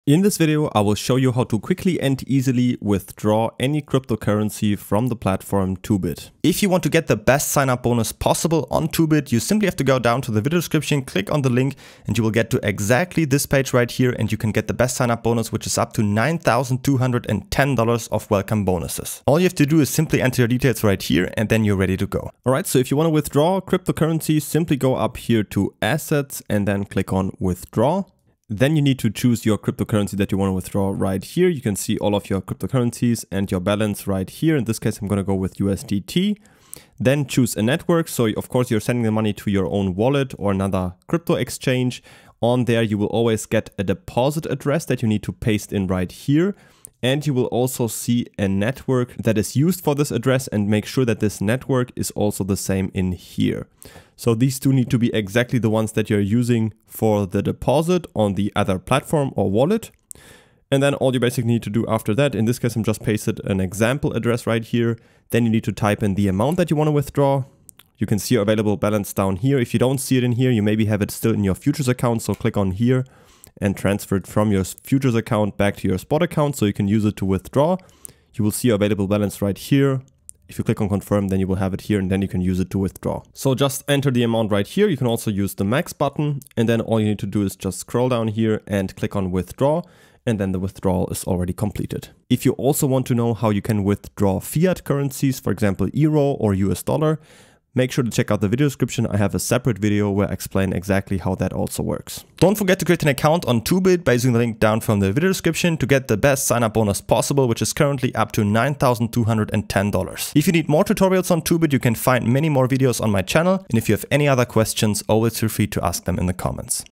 How to withdraw Crypto from Toobit ✅ Crypto Withdrawal Tutorial (Step-by-Step)
How to send Crypto from Toobit to your Wallet or another Exchange!

⏱️Timestamps⏱️
00:00 Create account (up to $9210 Bonus)
00:22 How to withdraw Crypto (BTC, USDT, ...)
01:42 Grab up to $9210 Bonus